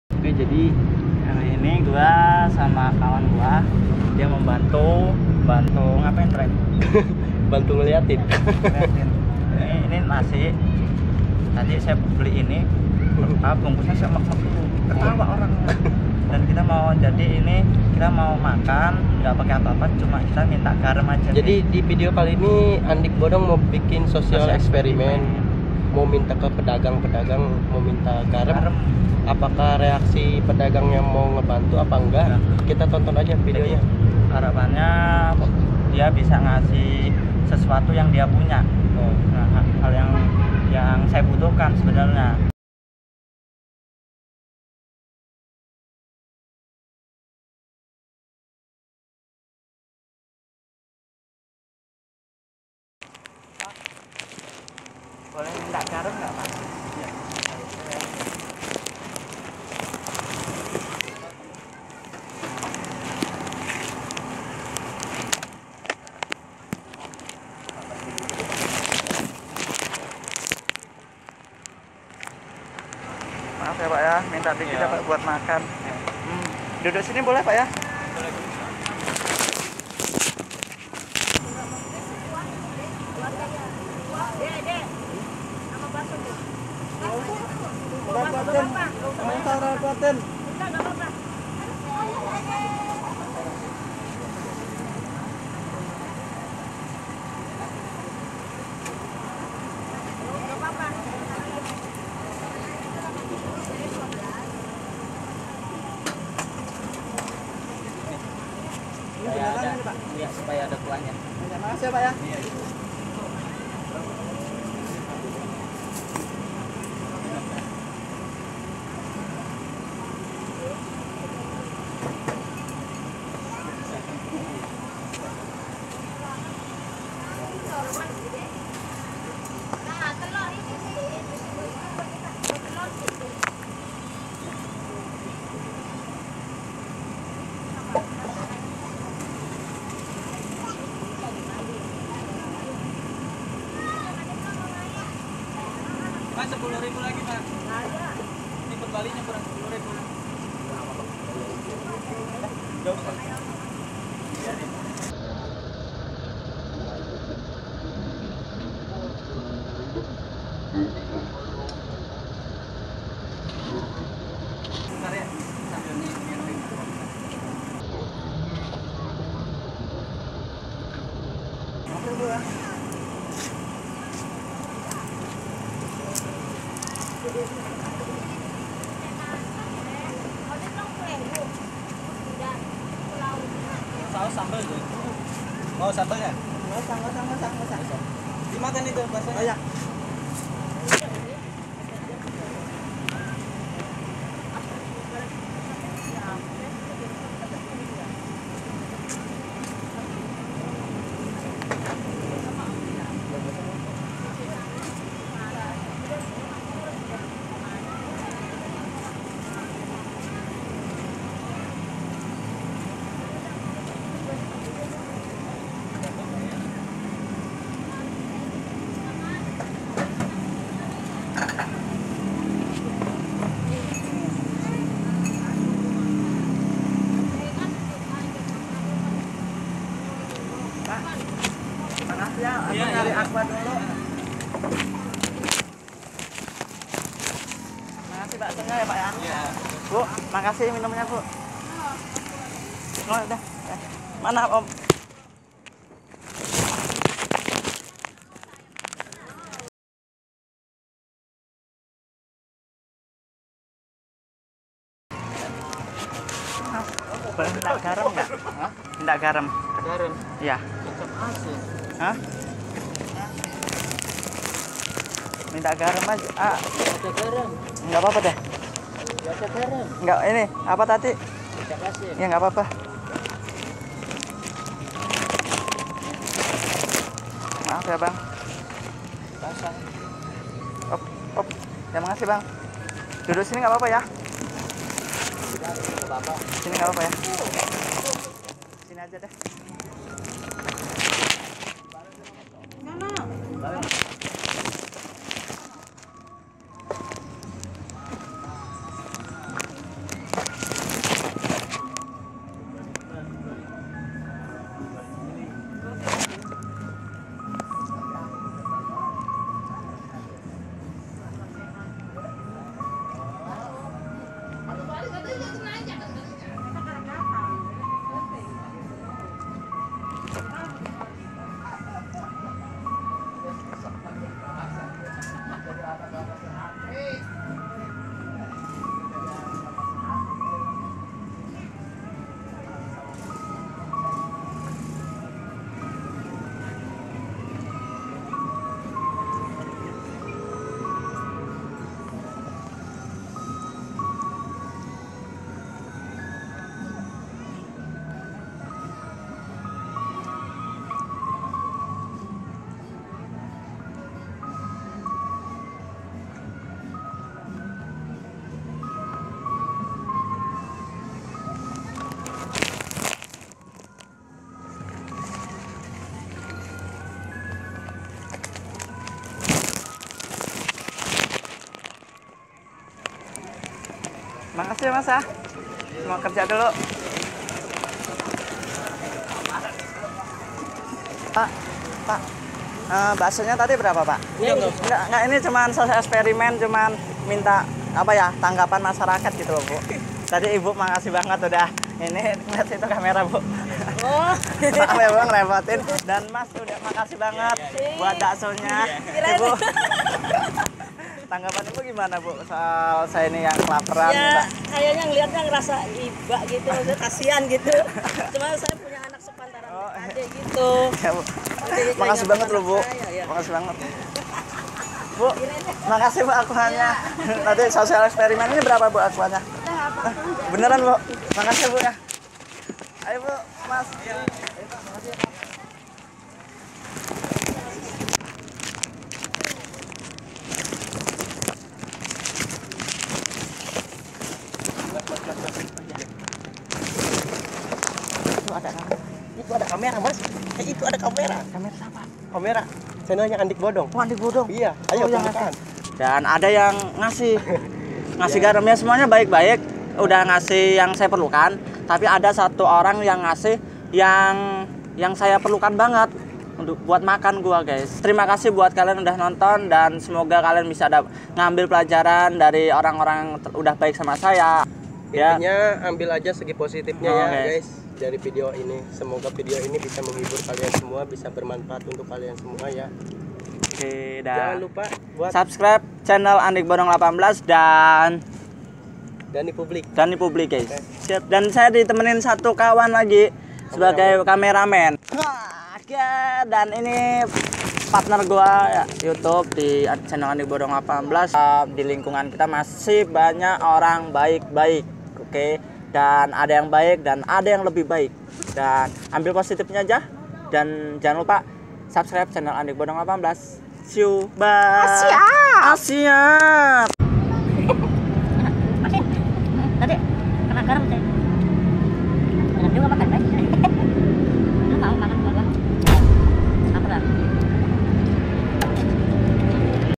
GEMBEL MINTAK GAREM BUAT MAKAN ,APAKAH TANGGAPAN MEREKA
OK kali ini kita buat video tentang gembel yang sedang kelaparan ddan ,iya hanya punya nasi dan tidak ada lauk  yang iya makan ,sehingga iya meminta garam buat pengganti lauk makan .
Gembel meminta garam ke pedagang yang ada di lingkungan terdekat .
bagaimana tanggapan penjual yang di mintai garam oleh gembel / Pemulung ,
Apakah Pedagang akan memberi sedikit garam yang di inginkan oeleh Gembel ,atau pedagang memberikan  Yang  Mereka punya  dan meberikan sesuatu yang terbaik mereka buat si Gembel ,berupa makanan.

                                     Dany publik